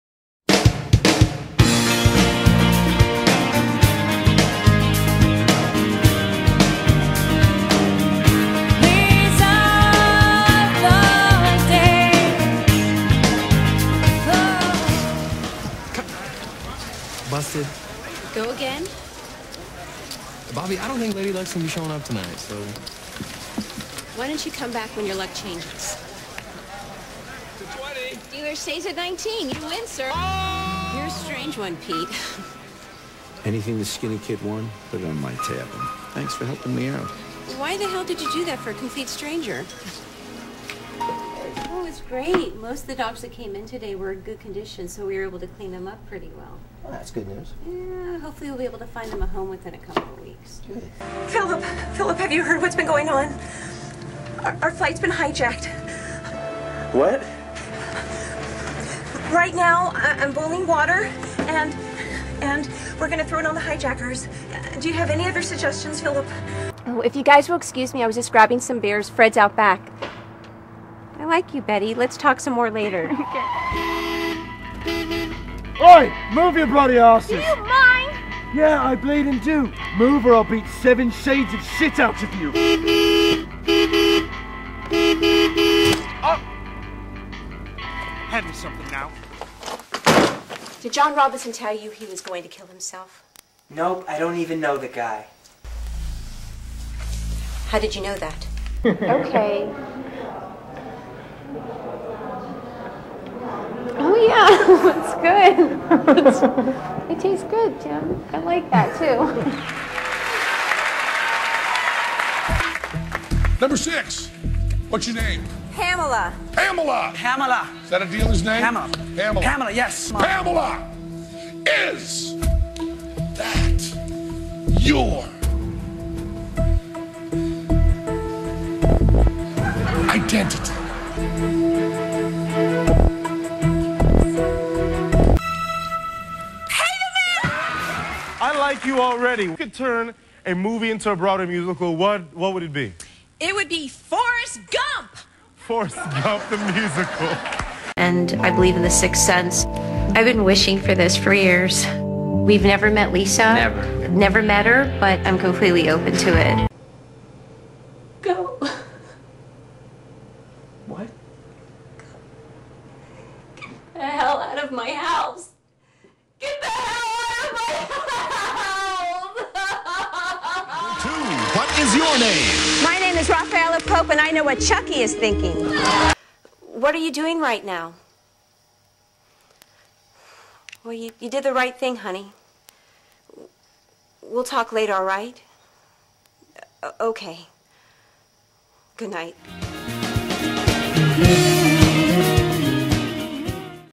Pamela Hill Acting Reel
Clips of Film & TV appearances with actress Pamela Hill.